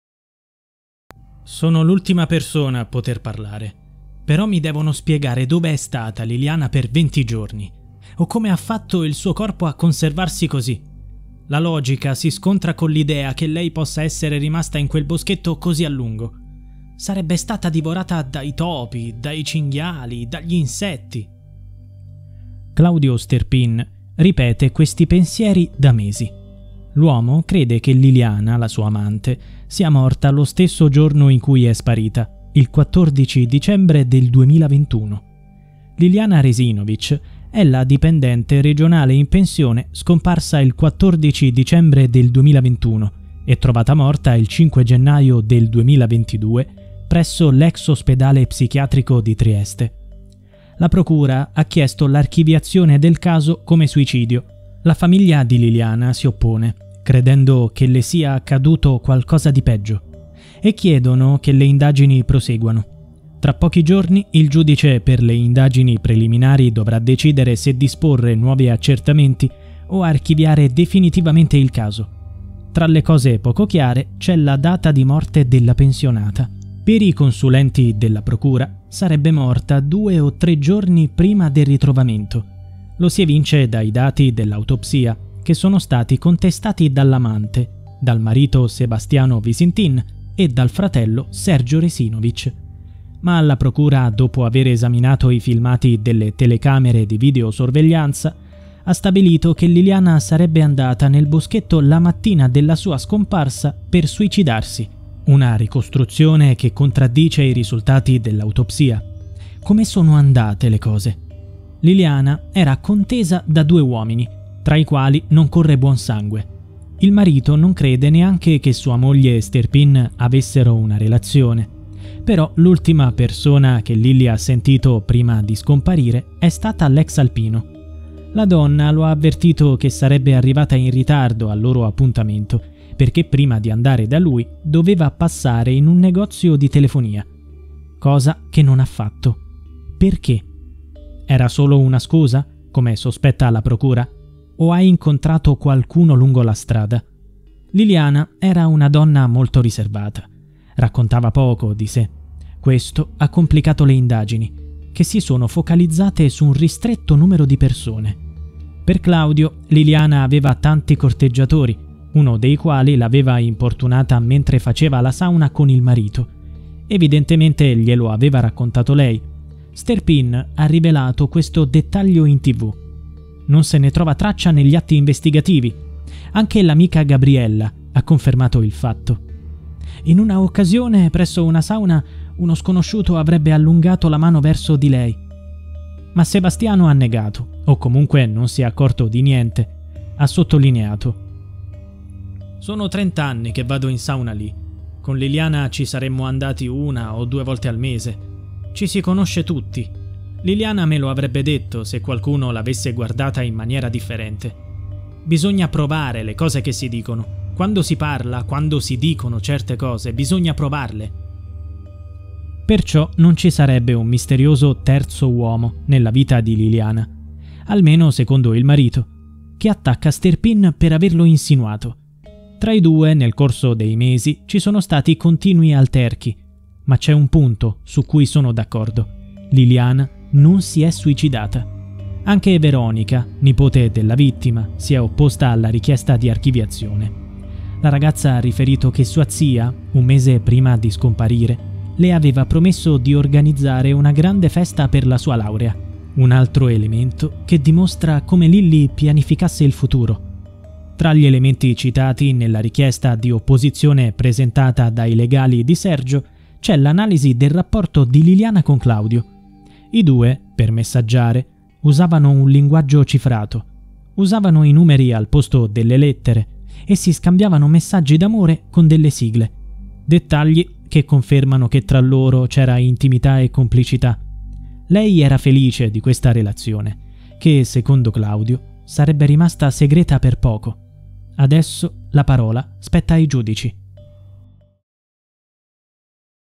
"Qualcuno la importunava" - Liliana Resinovich
Prosegue il giallo della morte di Liliana Resinovich